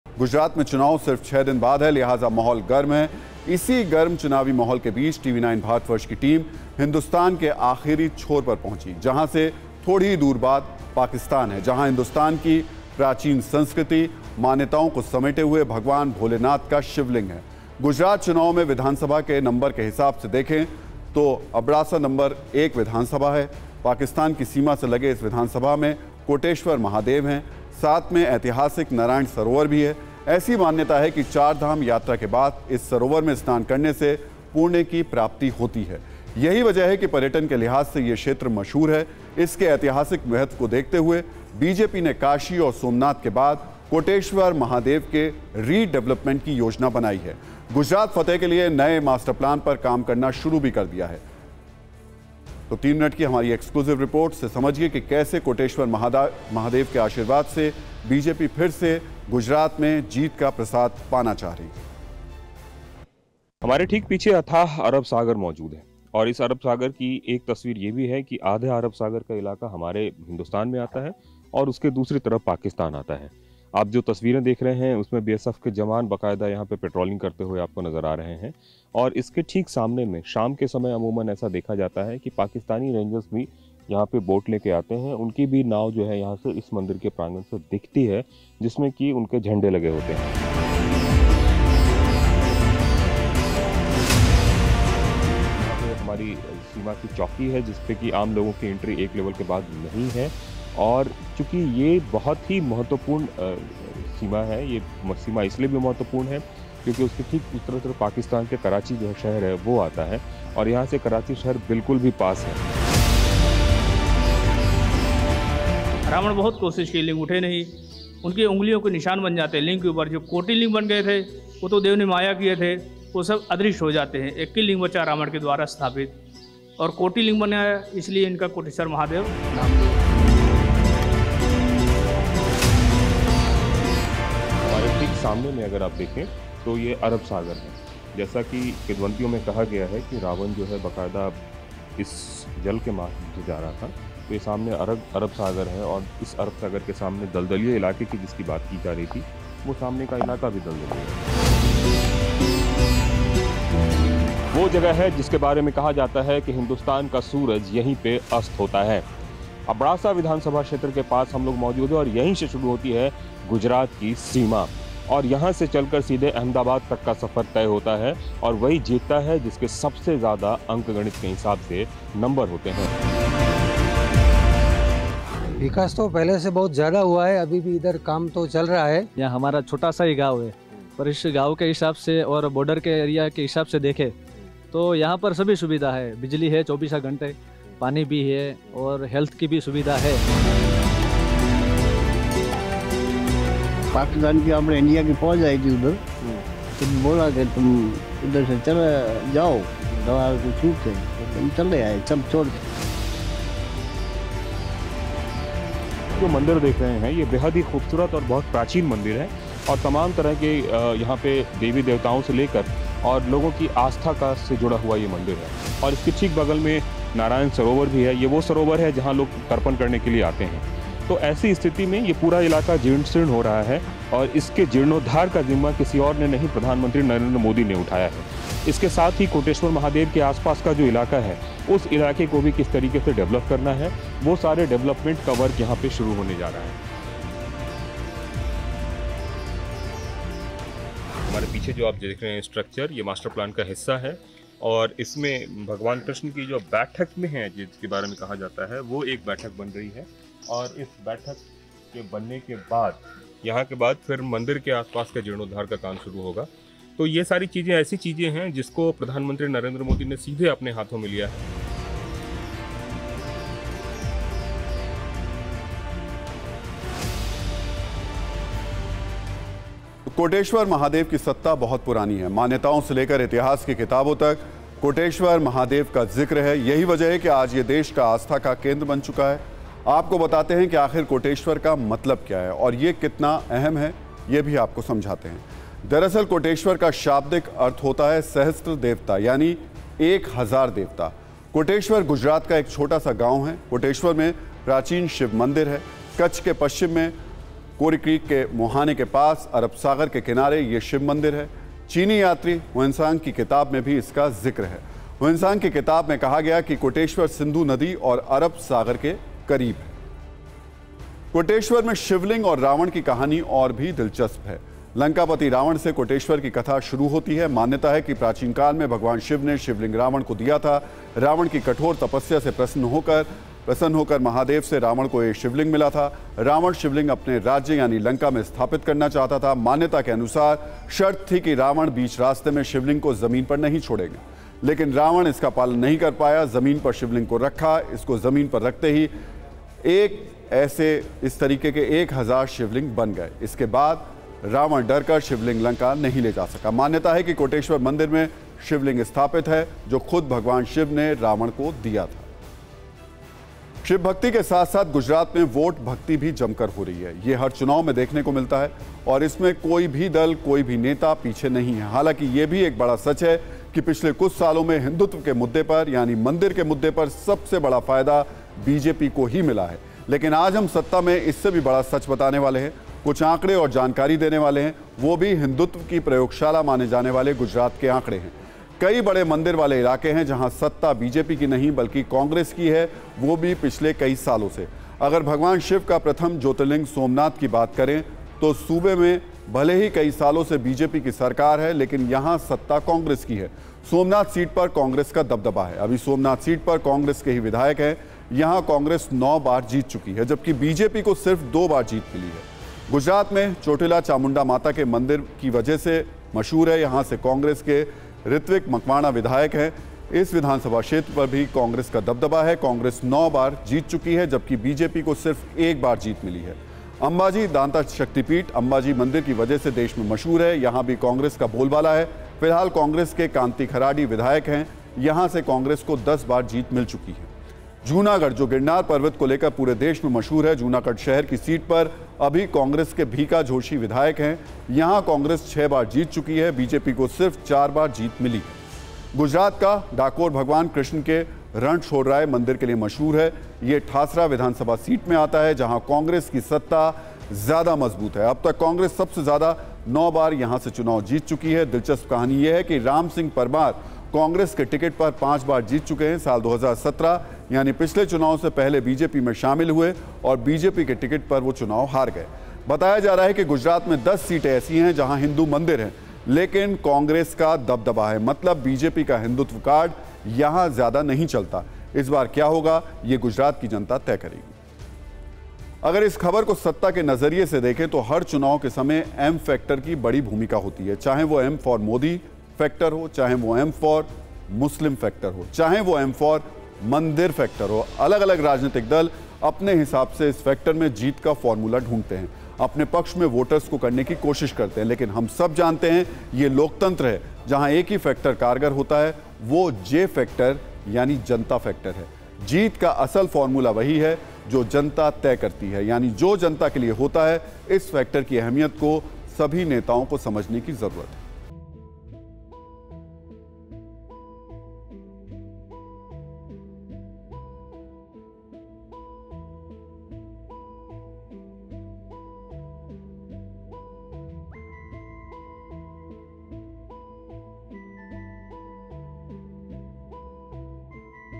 Gujarat Election 2022: BJP के हिंदुत्व कार्ड पर Congress भारी | Somnath Temple
Kashi..Somnath..Koteshwar..Gujarat का Winning Formula. Gujarat में मंदिर पॉलिटिक्स का महामुकाबला. गुजरात में सत्ता की कुर्सी..मंदिर के रास्ते !कोटेश्वर महादेव के आशीर्वाद से गुजरात विजय ! कोटेश्वर महादेव से EXCLUSIVE रिपोर्ट.